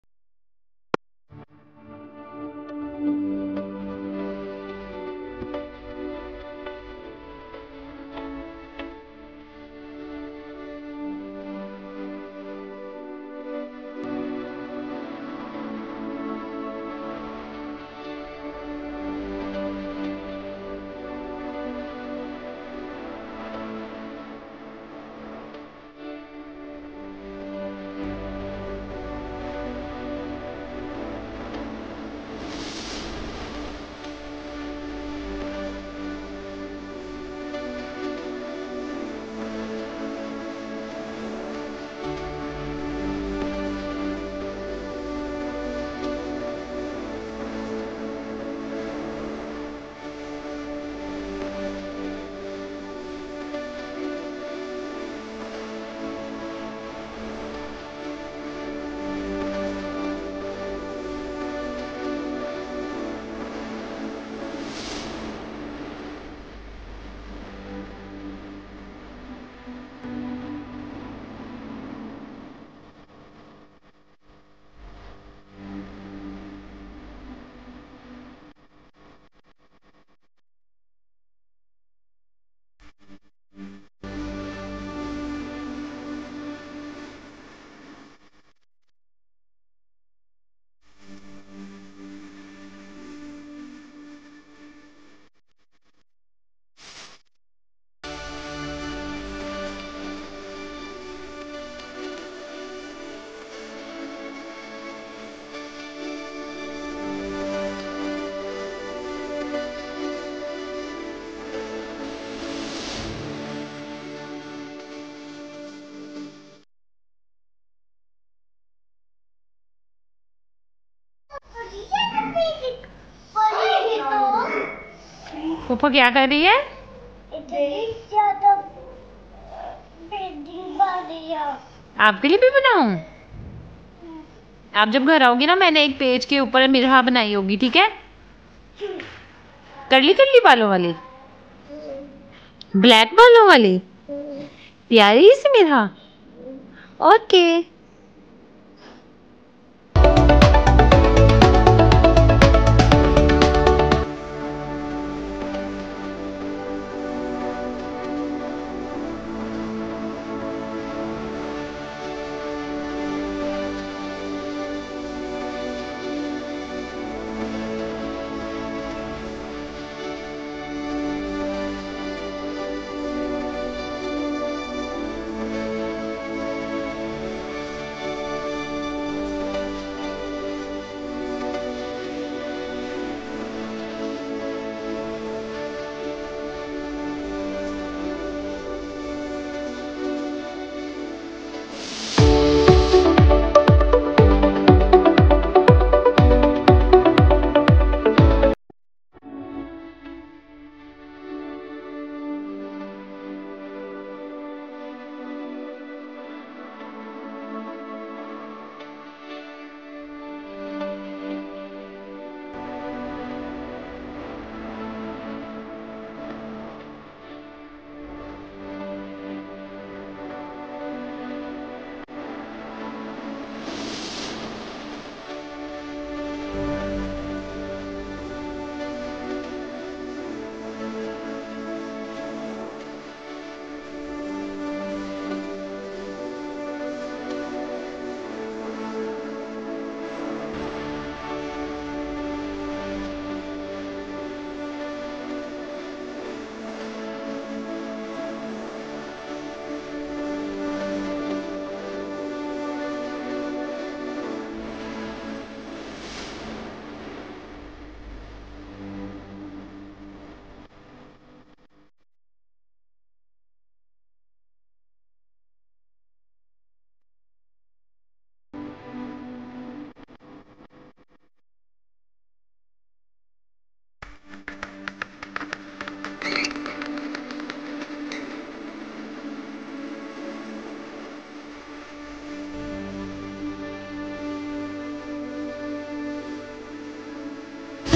Thankyou Card For My Friend ❤️
There is Nothing more beautiful than humanity and care ... So I try to make people happy who genuinely has good heart and here I made card for my friend at place work for a gesture of thanks ..
I am sharing it so that you people may get idea and make cute cards for your loved ones ...